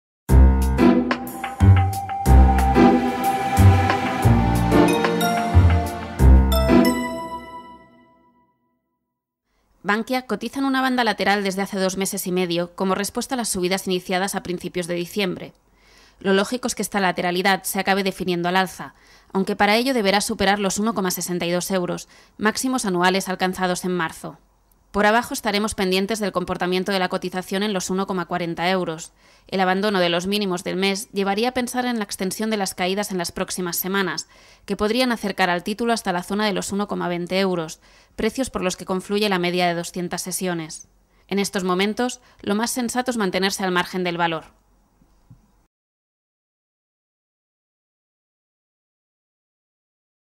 Análisis técnico de Bankia
Aunque lo más probable es que veamos la continuación de las subidas por debajo estaremos pendientes de su comportamiento en los 1,40 euros.

El abandono de los mínimos del mes nos haría pensar en la extensión de los descensos para las próximas semanas.